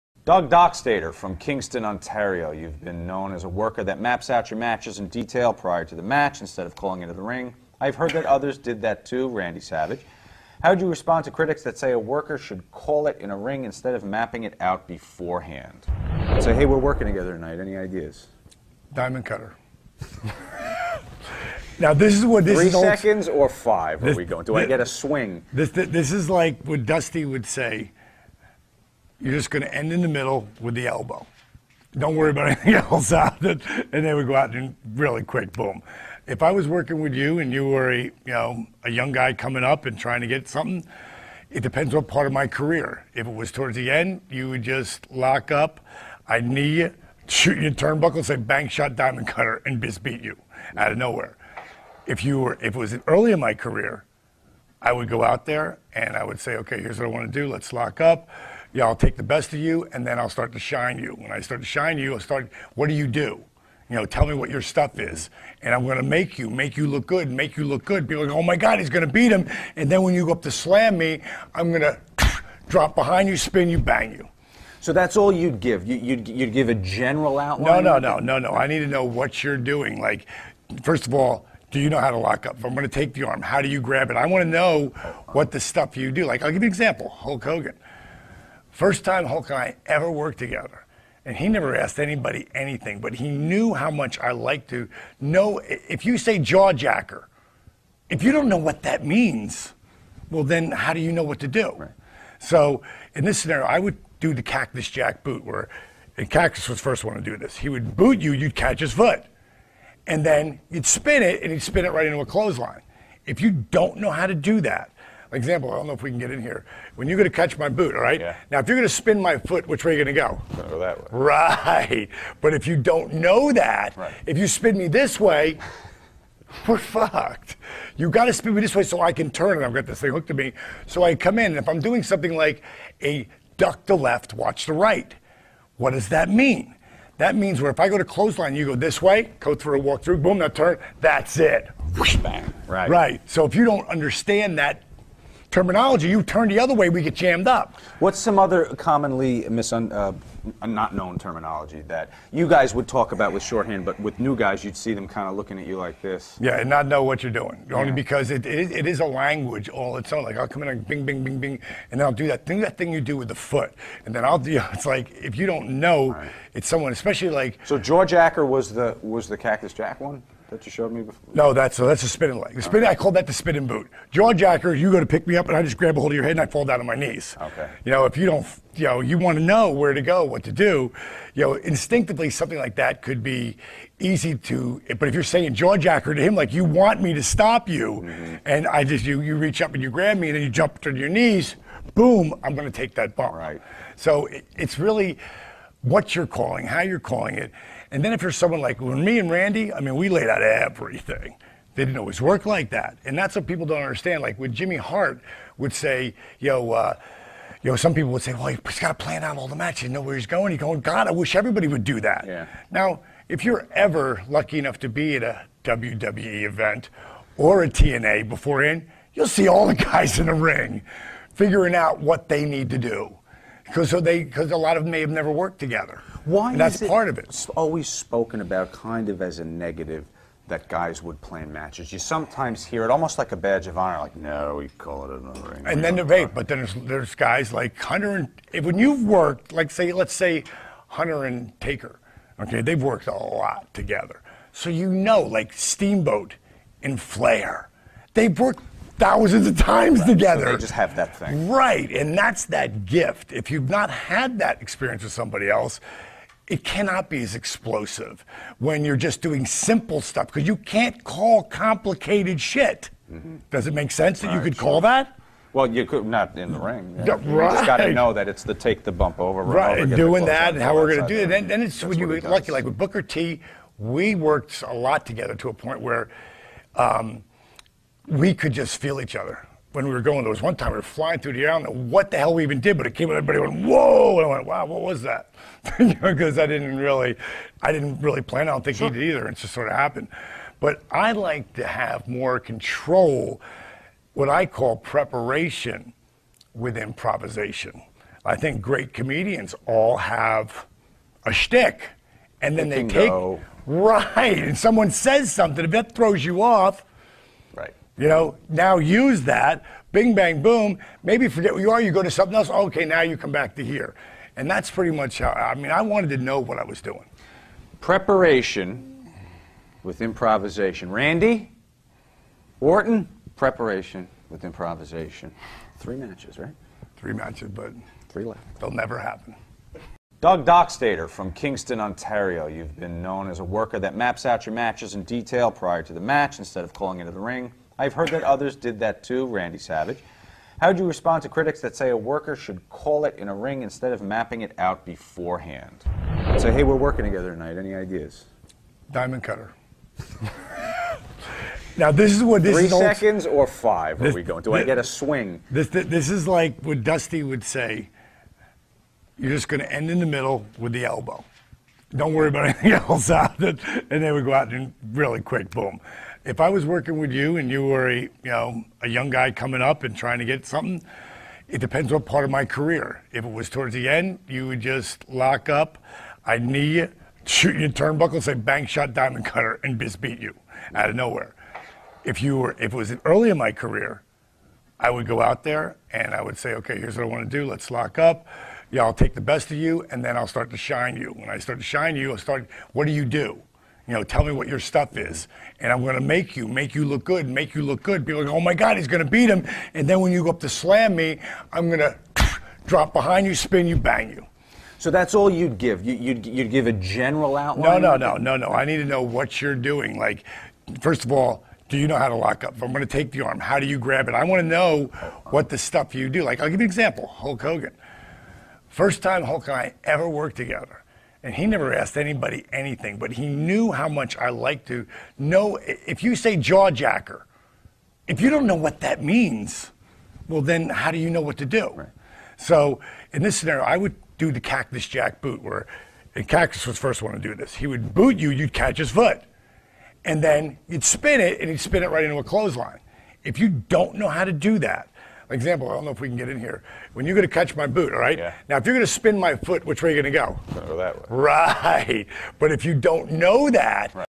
Diamond Dallas Page - Calling it in the ring vs mapping it out beforehand
it here: ...